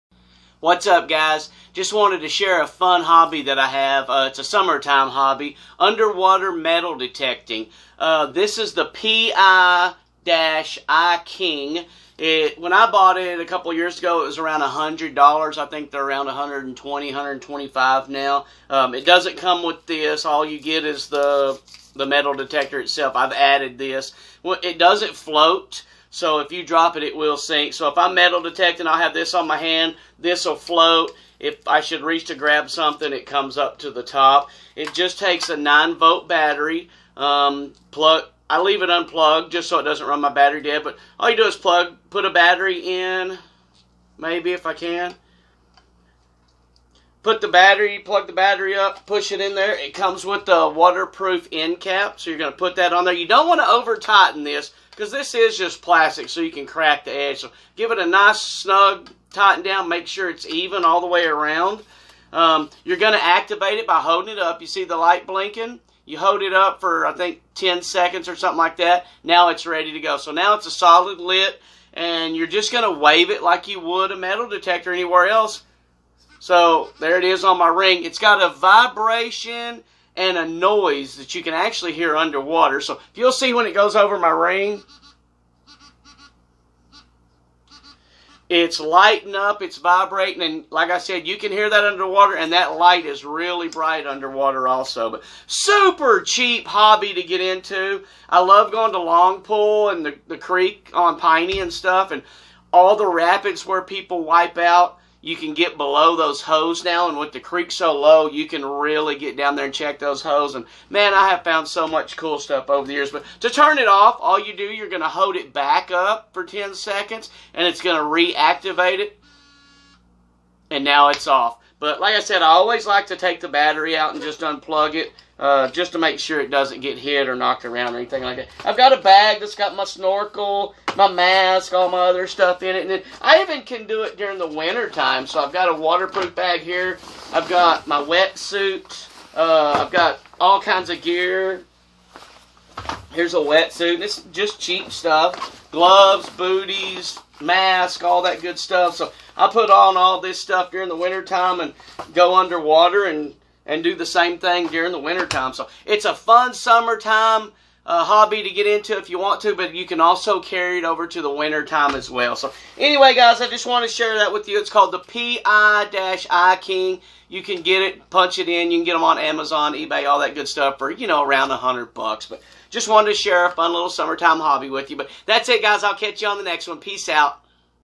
PI-iking underwater metal detector review.
Looking for a fun, inexpensive summertime hobby, try underwater metal detecting. It only costs around $100 to get into and you can find some really cool stuff. Check out the video I did on this detector and see if it's something you might be interested in.